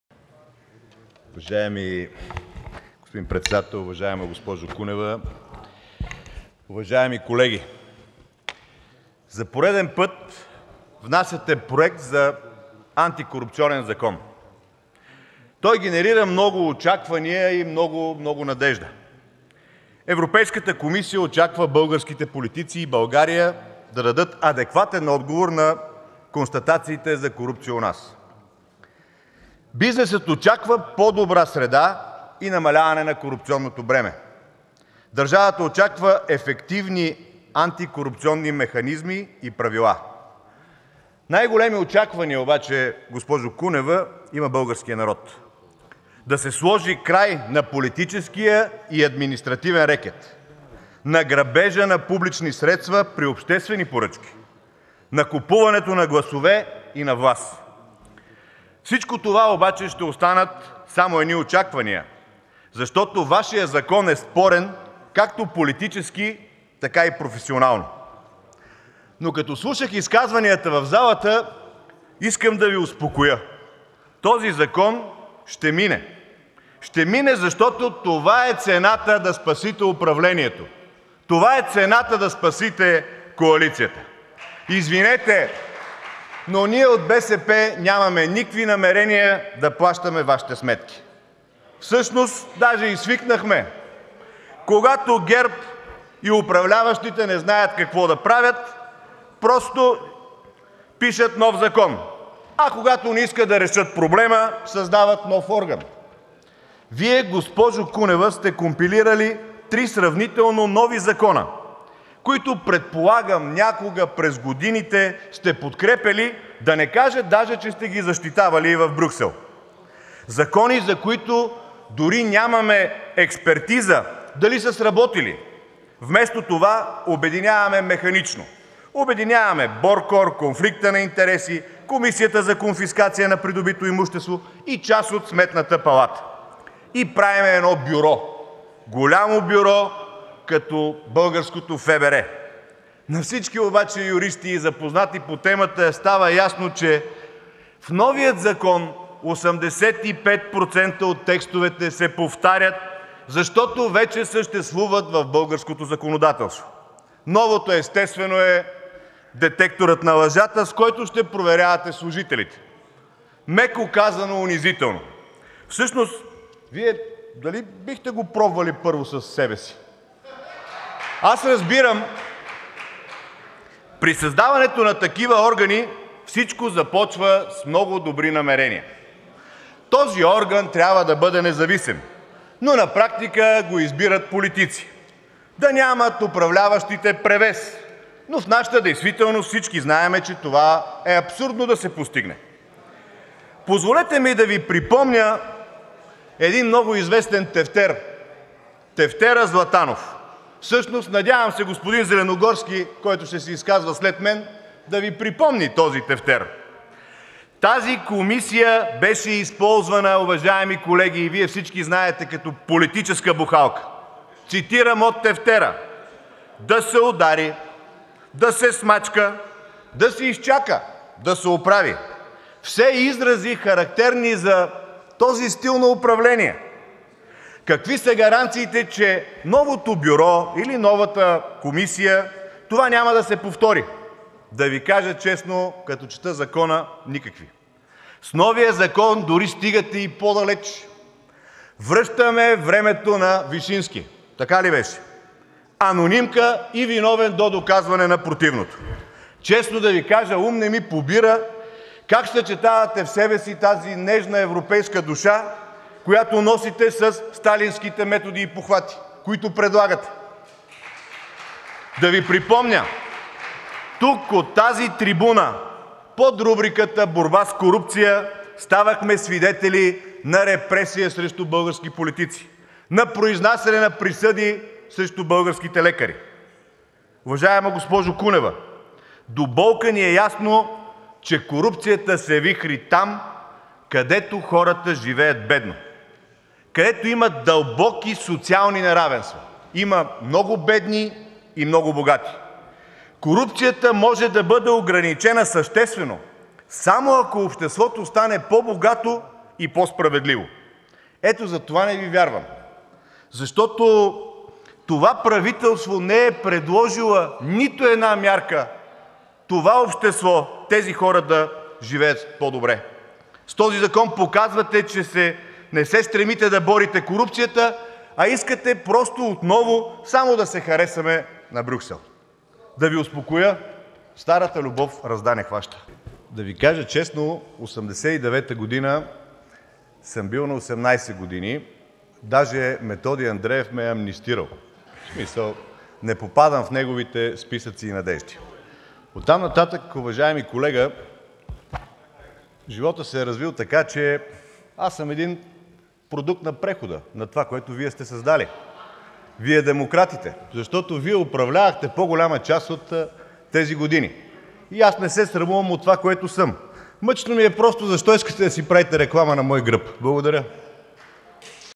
КИРИЛ ДОБРЕВ В НС НА 30.06.2016 "Антикорупционният закон е цената за спасяване на кабинета"
„Антикорупционният закон на министър Кунева е спорен, както политически, така и професионално, но ще мине, защото това е цената да се спаси управляващата коалиция.  Извинете, но ние от БСП нямаме никакво намерение да ви плащаме сметките”. Това заяви от парламентарната трибуна народният представител от ПГ на БСП ЛЯВА БЪЛГАРИЯ и член на ИБ на НС на БСП Кирил Добрев.